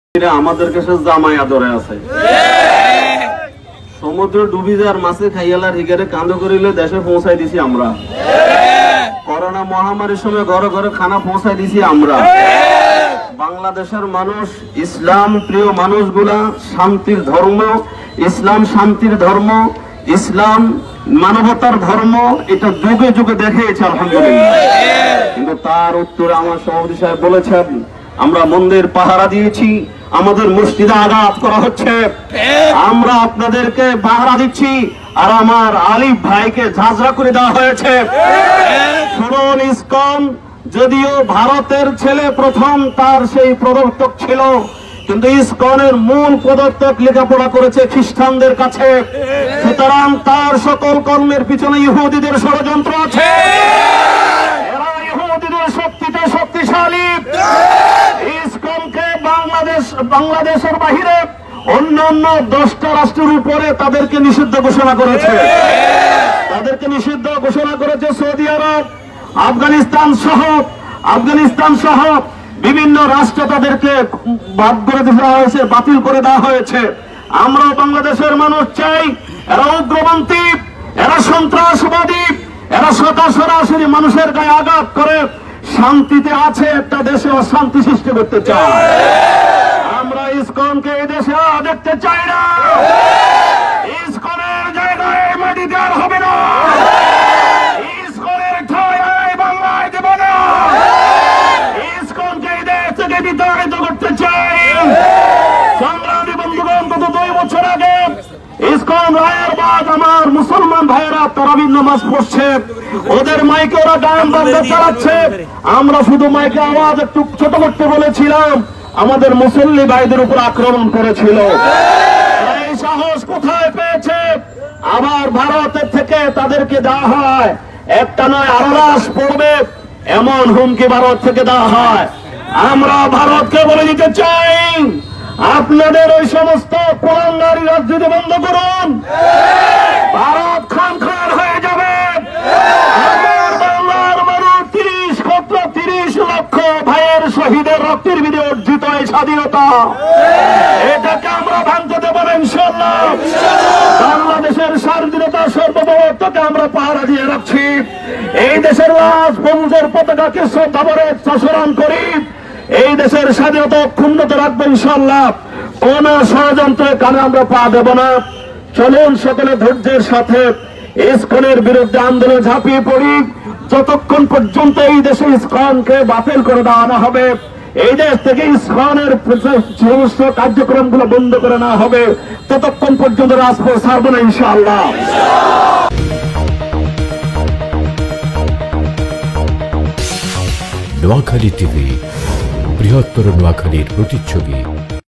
সেনবাগ বাজারে এ্যাডভোকেট সাইফুল ইসলাম আলিফ হ ত্যা ও ই স ক ন-নি ষি দ্ধে র প্রতিবাদে Noakhali tv
সেনবাগ বাজারে এ্যাডভোকেট সাইফুল ইসলাম আলিফ হ ত্যা ও ই স ক ন-নি ষি দ্ধে র প্রতিবাদে
বিক্ষোভ মিছিল অনুষ্ঠিত হয়।